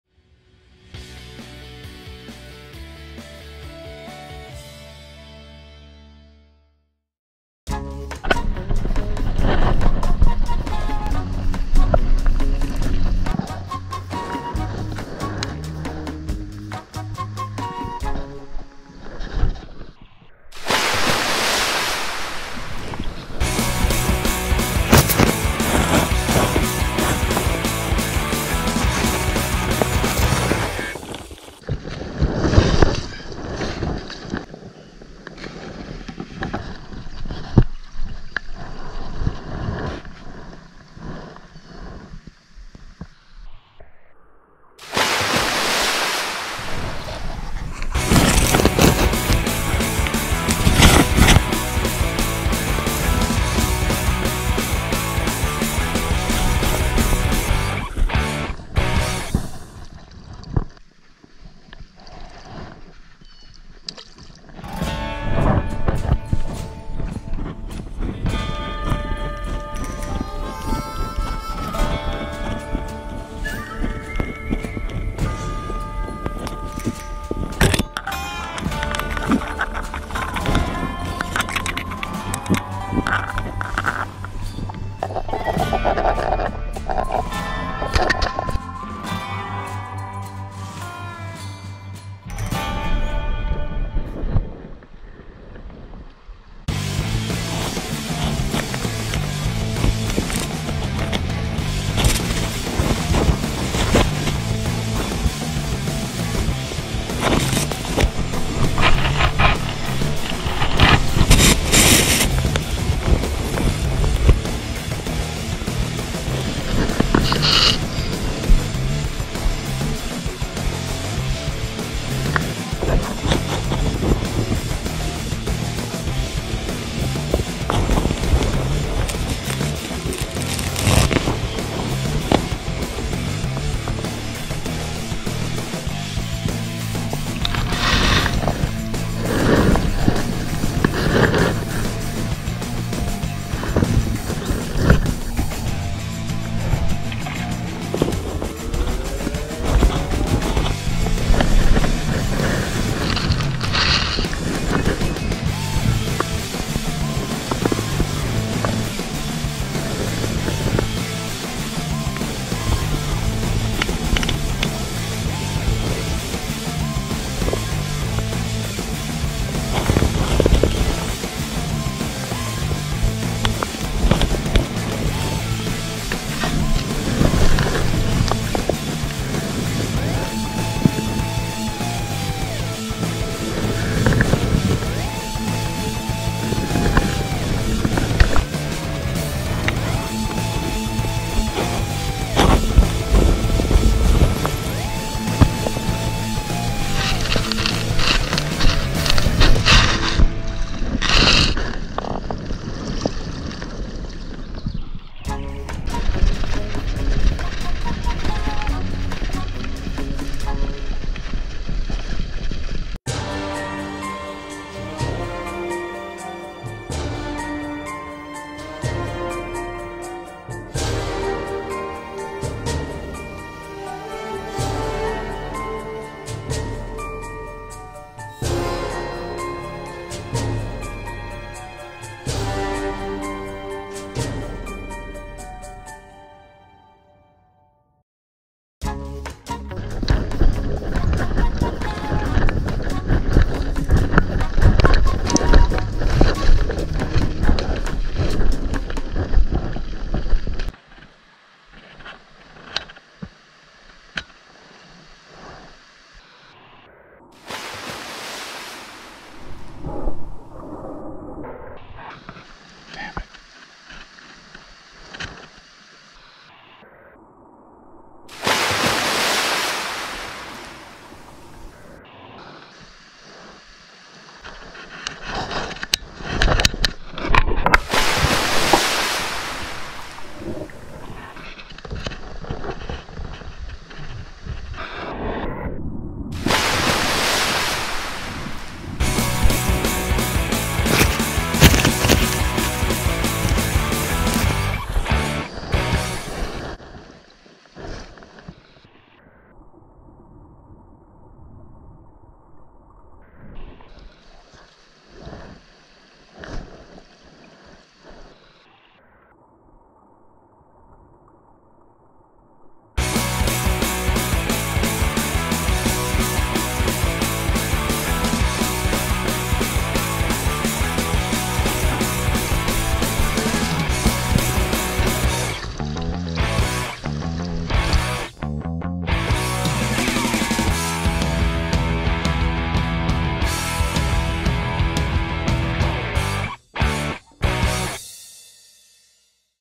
Incredible Fly Fishing on Spring Creek
We had an incredible time fishing for wild Brown Trout on Spring Creek in Pennsylvania.  A few were landed on streamers but mostly dry flies and indicator nymphing.